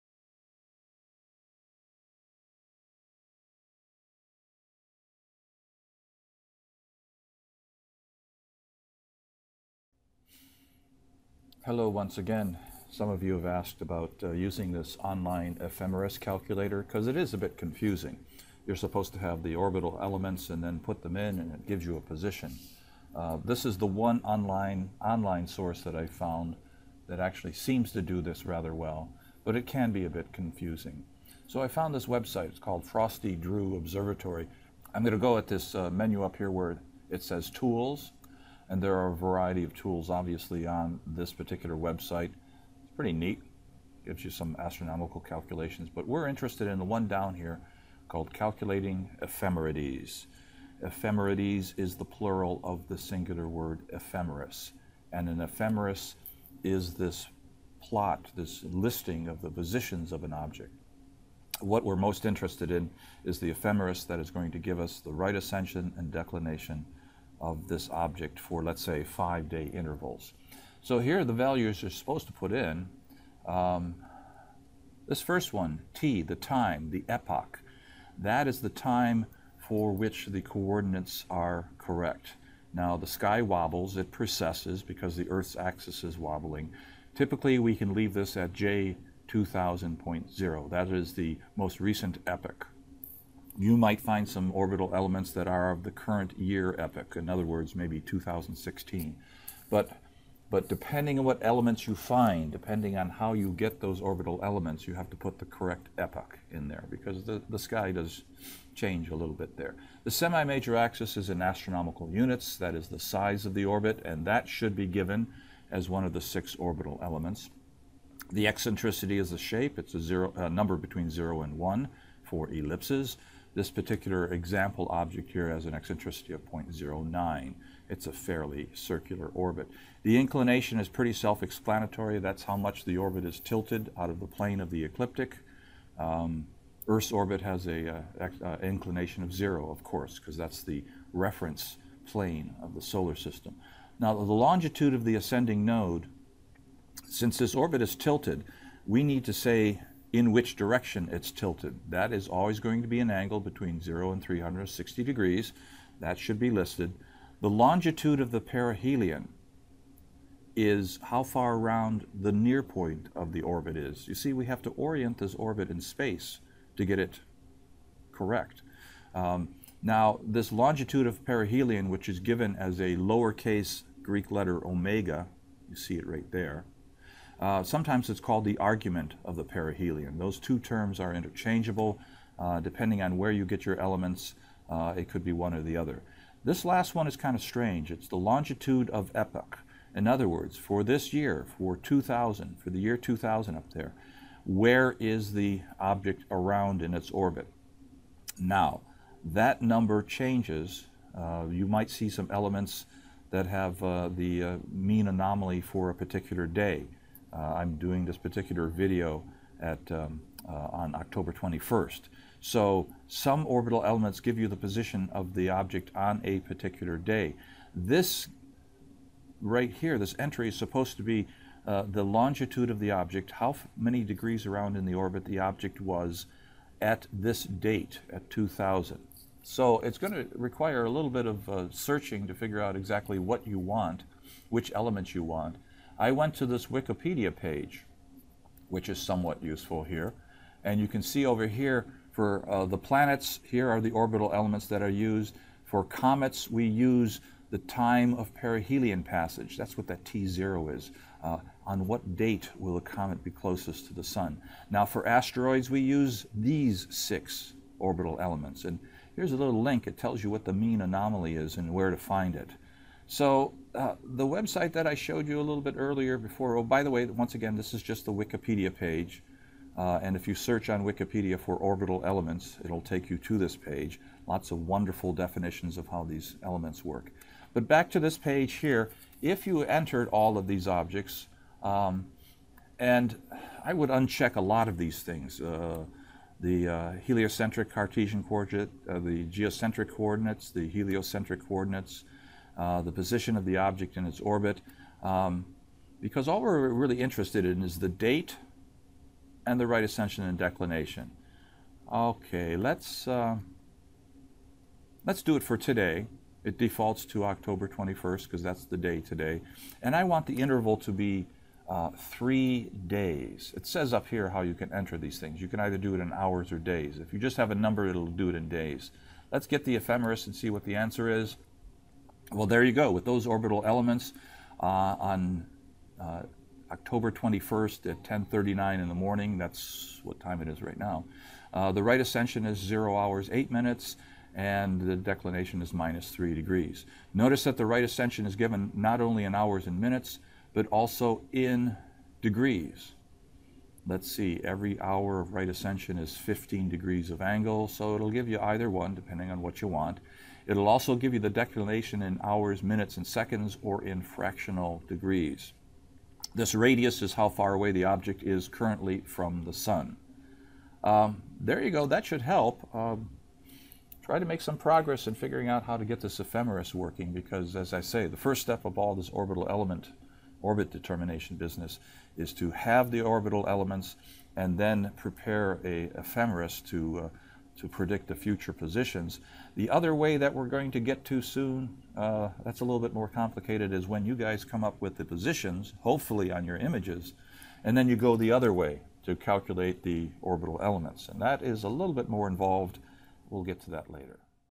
Calculating Ephemerides Tutorial
This tutorial explains how to use the online resource at the Frosty Drew Observatory website.